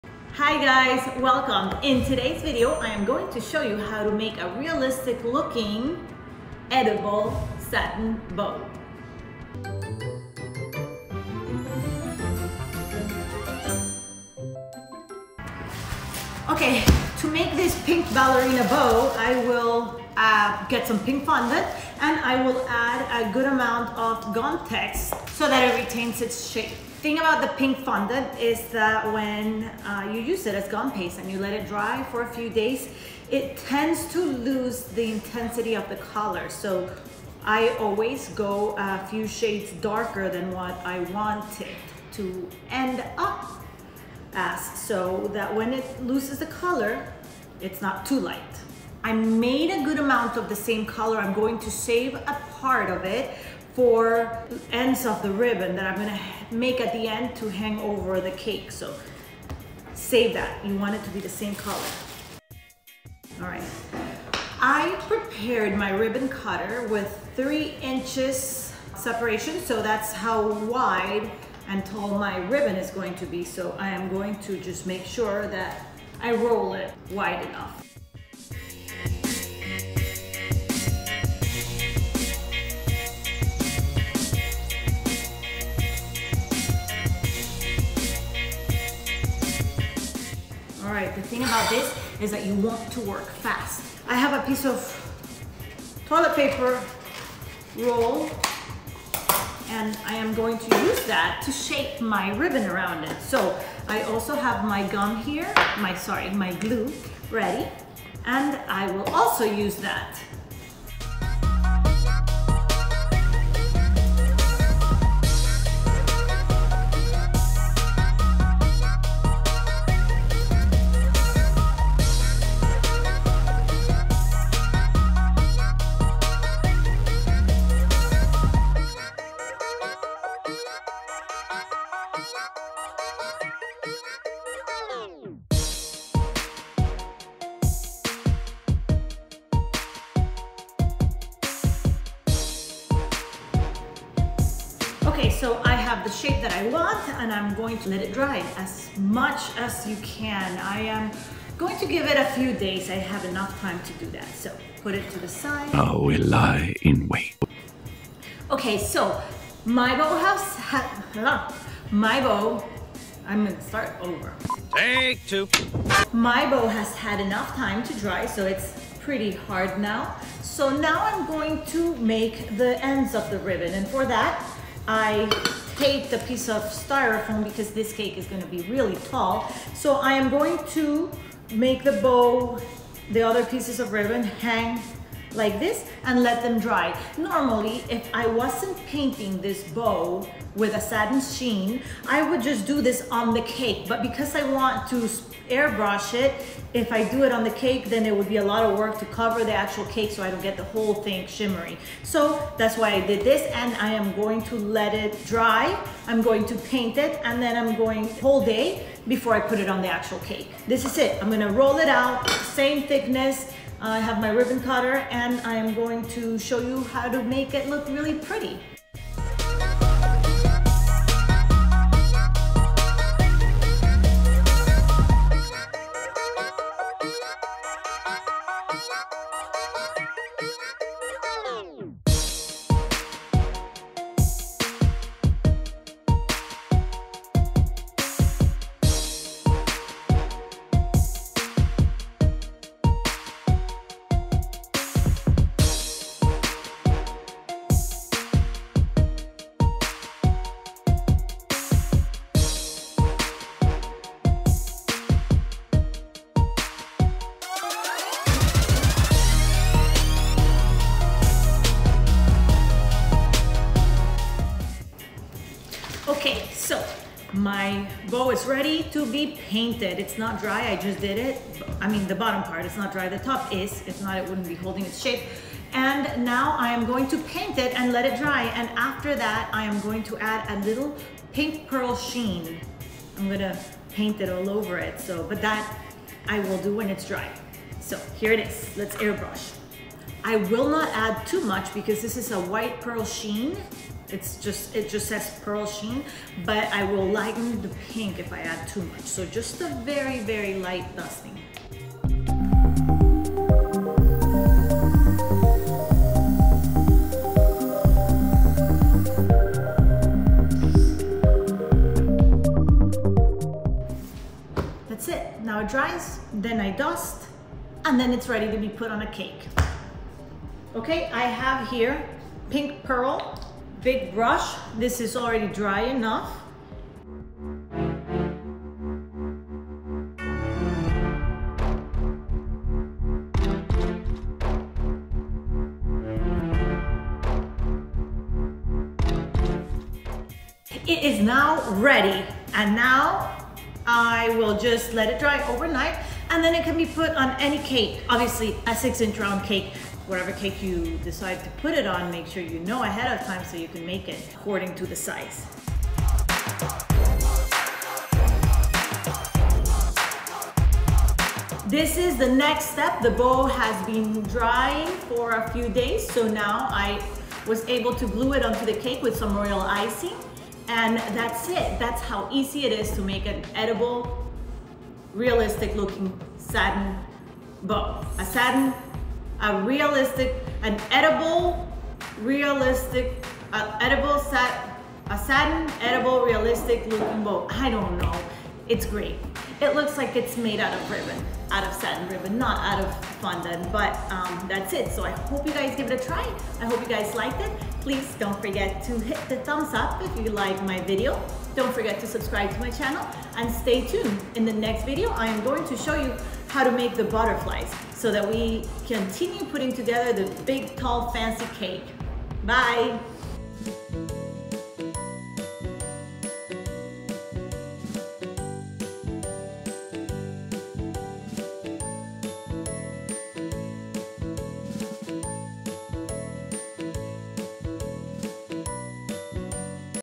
How To Make A Beautiful Fondant Bow Cake Topper | We Heart Cake
Here make a Fondant Bow to put on top of a cake. I hope you find this video helpful.

The thing you need:
Rice Krispies Treats